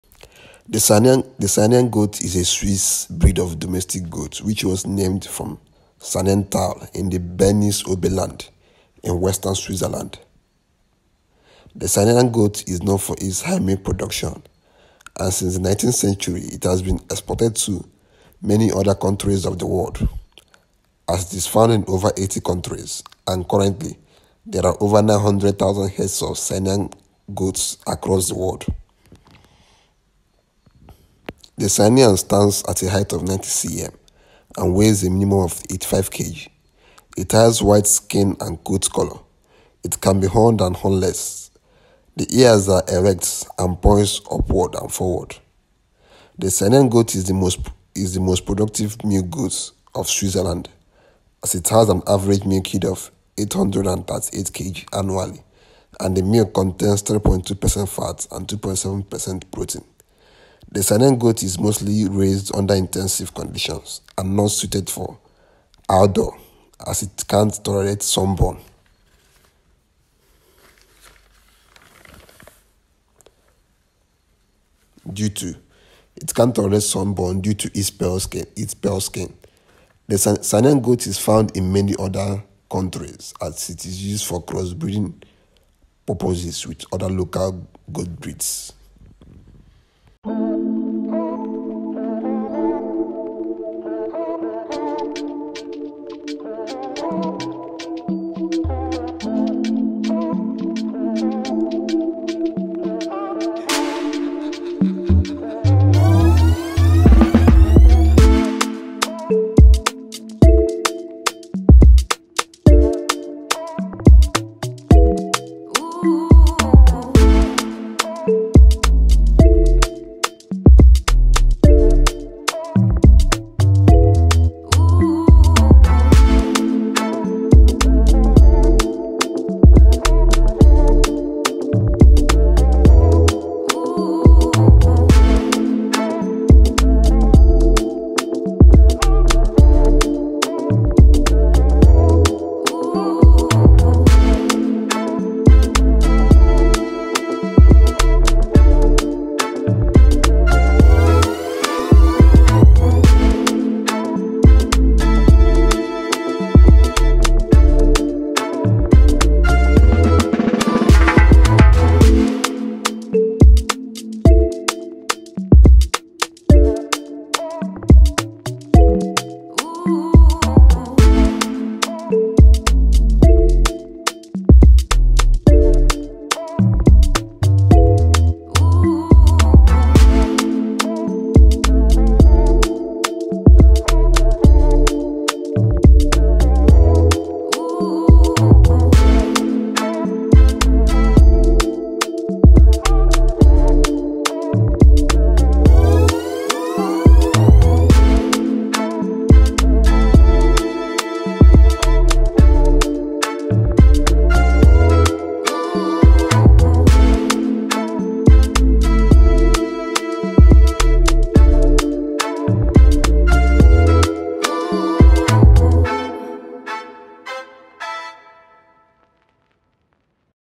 Saanen Goat | Facts, History & Characteristics
The Saanen, German: 'Saanenziege', French: 'Chèvre de Gessenay', is a Swiss breed of domestic goat. It takes its name from the Saanental in the Bernese Oberland, in the southern part of the Canton of Bern, in western Switzerland